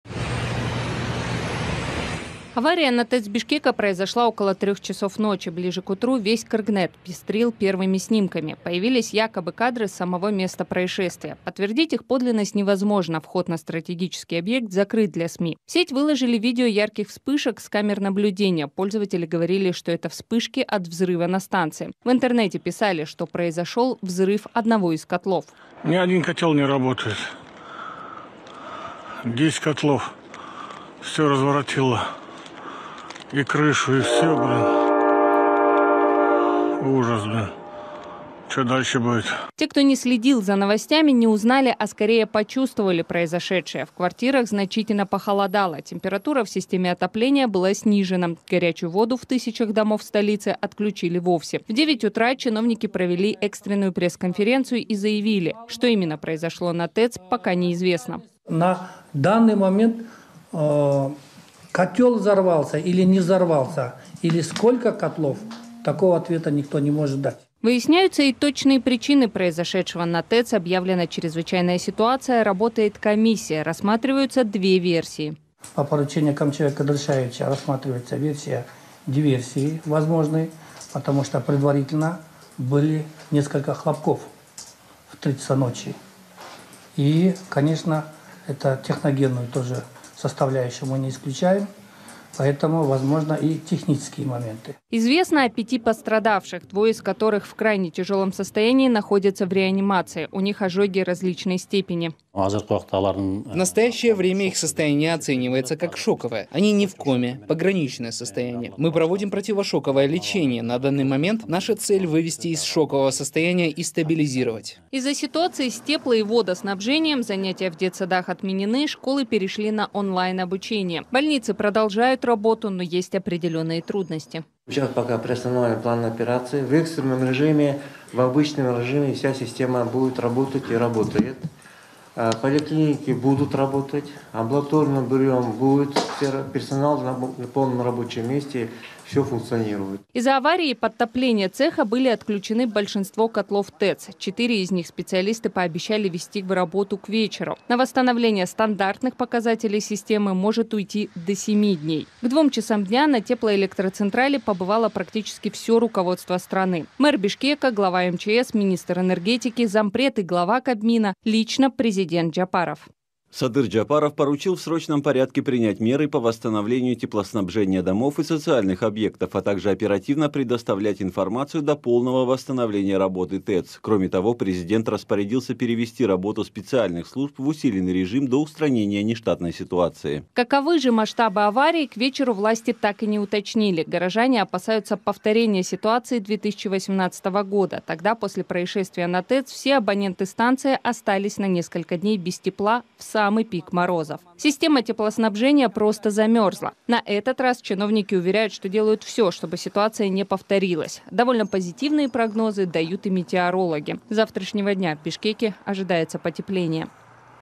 Крупная авария на ТЭЦ Бишкека
Крупная авария на ТЭЦ в столице Кыргызстана. В Бишкеке большинство жителей остались без горячей воды, а часть многоквартирных домов – без тепла. Власти не исключают диверсию на стратегическом объекте.